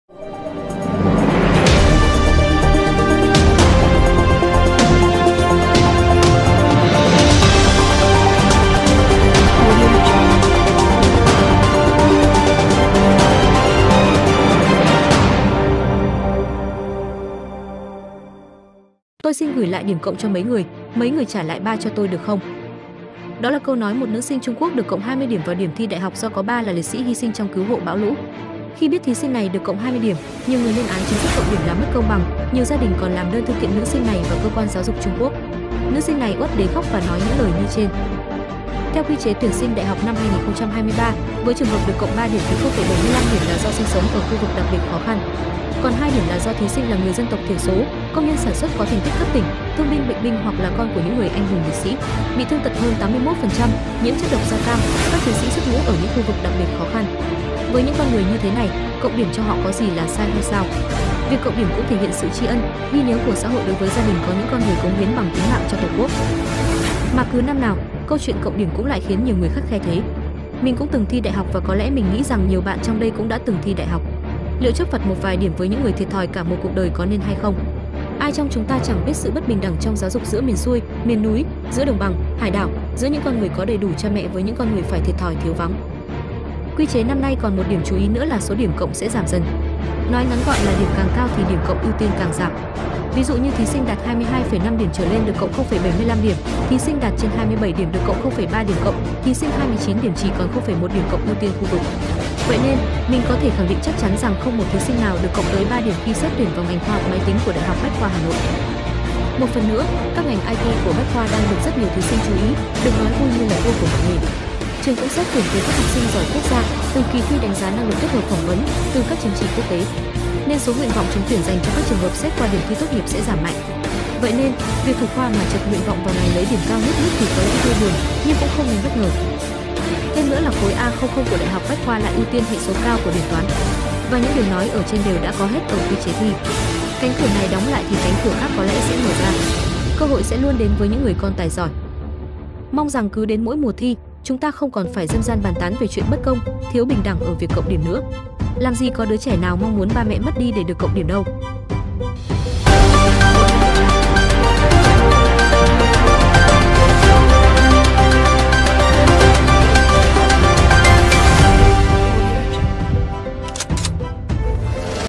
TÔI XIN GỬI LẠI ĐIỂM CỘNG CHO MẤY NGƯỜI, MẤY NGƯỜI TRẢ LẠI BA CHO TÔI ĐƯỢC KHÔNG?
Đó là câu nói một nữ sinh Trung Quốc được cộng 20 điểm vào điểm thi đại học do có ba là liệt sĩ hy sinh trong cứu hộ bão lũ. Khi biết thí sinh này được cộng 20 điểm, nhiều người lên án chính sách cộng điểm là mất công bằng, nhiều gia đình còn làm đơn thư kiện nữ sinh này và cơ quan giáo dục Trung Quốc. Nữ sinh này uất đến khóc và nói những lời như trên.
Theo quy chế tuyển sinh đại học năm 2023, với trường hợp được cộng 3 điểm thì 0,75 điểm là do sinh sống ở khu vực đặc biệt khó khăn. Còn 2 điểm là do thí sinh là người dân tộc thiểu số, công nhân sản xuất có thành tích cấp tỉnh, thương binh bệnh binh hoặc là con của những người anh hùng liệt sĩ, bị thương tật hơn 81%, nhiễm chất độc da cam, các chiến sĩ xuất ngũ ở những khu vực đặc biệt khó khăn… Với những con người như thế này, cộng điểm cho họ có gì là sai hay sao? Việc cộng điểm cũng thể hiện sự tri ân, ghi nhớ của xã hội đối với gia đình có những con người cống hiến bằng tính mạng cho Tổ Quốc.